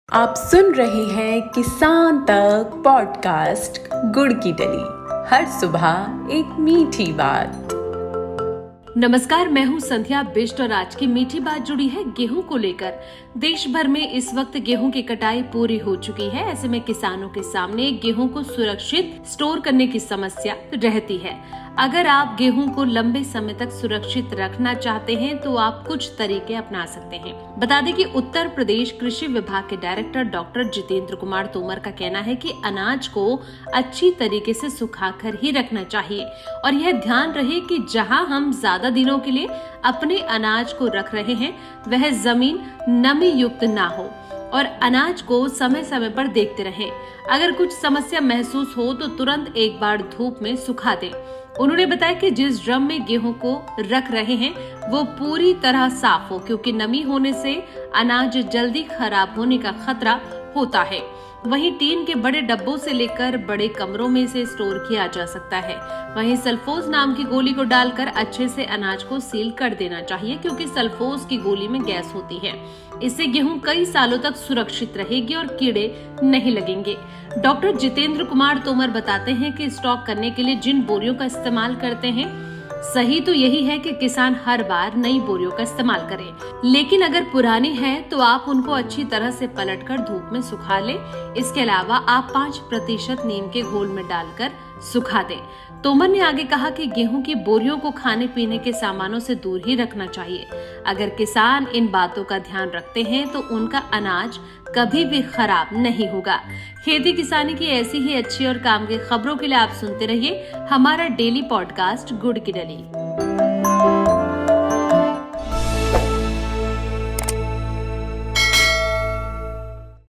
Podcast: क्या आपका गेहूं भी हो रहा है खराब तो समय रहते कर लें ये काम, मिलेगा फायदा | Kisan Tak
इस समय यूपी में गेहूं की कटाई का काम पूरी तरह से खत्म हो चुका है. अब ऐसे में हर किसान के पास एक ही चिंता सता रही है कि आखिरकार वो कैसे गेहूं को अपने घर में ज्यादा समय तक सुरक्षित रख सके. इस मामले में किसान तक ने उत्तर प्रदेश कृषि विभाग के डायरेक्टर डॉ जितेंद्र कुमार तोमर से खास बातचीत की. डॉ तोमर कई ऐसे टिप्स बताए जिससे किसान अपनी गेहूं को कई सालों तक घर और गोदाम में सुरक्षित रख सकता हैं.